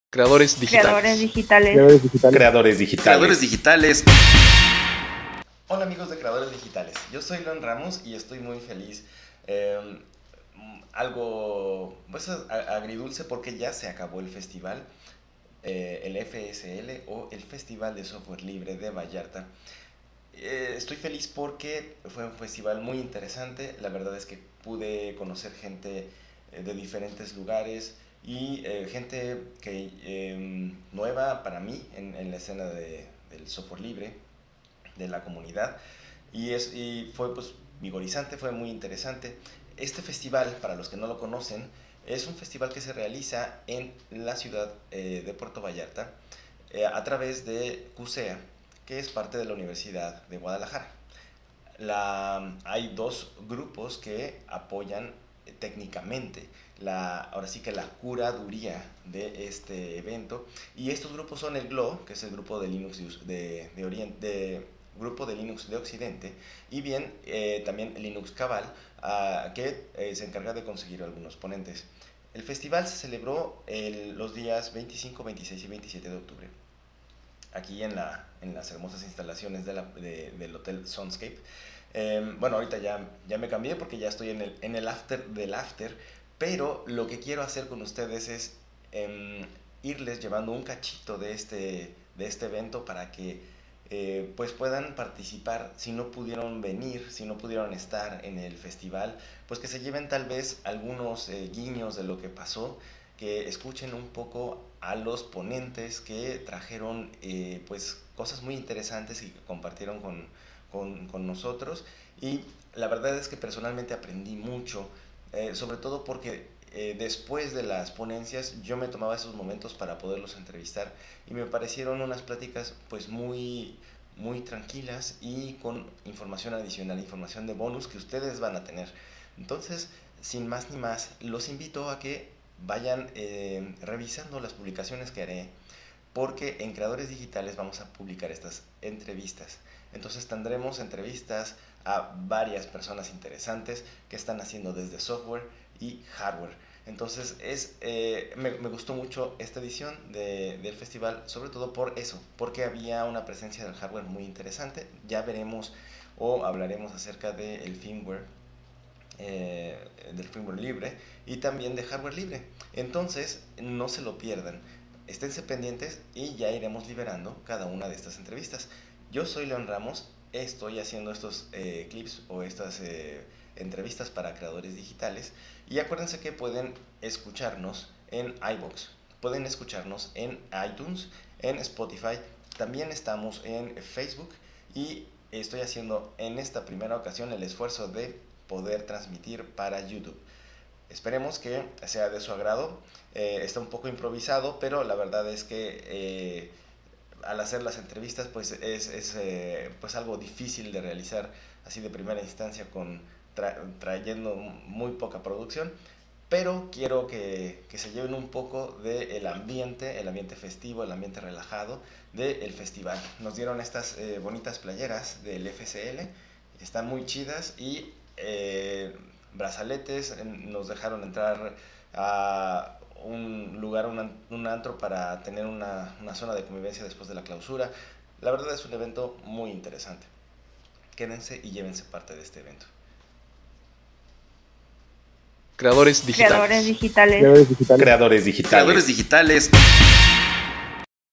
Nuestra cobertura del FSL Vallarta 2019
El Festival de Software Libre de Vallarta es un evento icónico en la comunidad de software libre, con ya 18 años de existencia.

Me di a la tarea de seguir a los ponentes y robarles unos minutos para varias entrevistas. Nos compartieron lo que presentaron en el FSL y algunas anécdotas personales muy interesantes.

No se pierdan las entregas que iremos liberando semana a semana.

CREADORES DIGITALES

Síguenos!

Creative Commons Audios Used